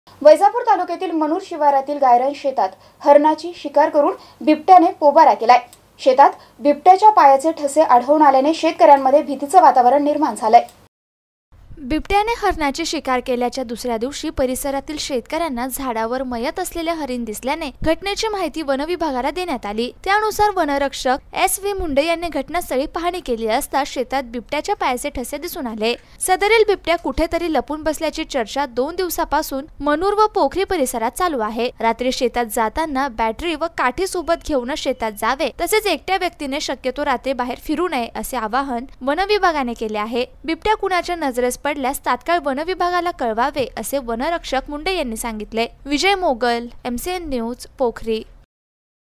वैजापूर तालुक्यातील मणुर शिवारातील गायरान शेतात हरणाची शिकार करून बिबट्याने पोबारा केला आहे
# MCN News # Aurangabad # Marathwada